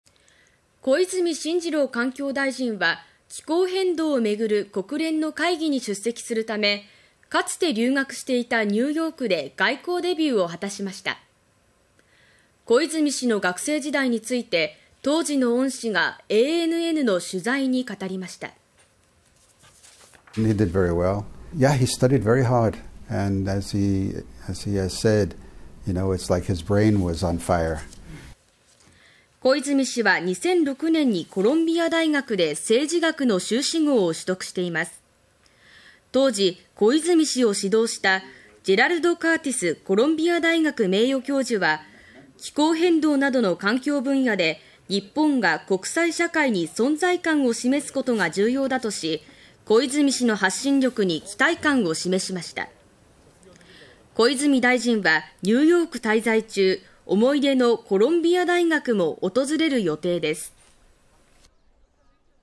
米・コロンビア大学の恩師 小泉大臣の学生時代語る(19/09/23)
小泉進次郎環境大臣は気候変動を巡る国連の会議に出席するため、かつて留学していたニューヨークで外交デビューを果たしました。小泉氏の学生時代について、当時の恩師がANNの取材に語りました。

　コロンビア大学、ジェラルド・カーティス名誉教授：「彼は成績も良かったし、一生懸命、勉強した。『頭から火が出そうだった』と本人も言っていたよ」
　小泉氏は2006年にコロンビア大学で政治学の修士号を取得しています。当時、小泉氏を指導したコロンビア大学・カーティス名誉教授は気候変動などの環境分野で日本が国際社会に存在感を示すことが重要だとし、小泉氏の発信力に期待感を示しました。小泉氏はニューヨーク滞在中、思い出のコロンビア大学も訪れる予定です。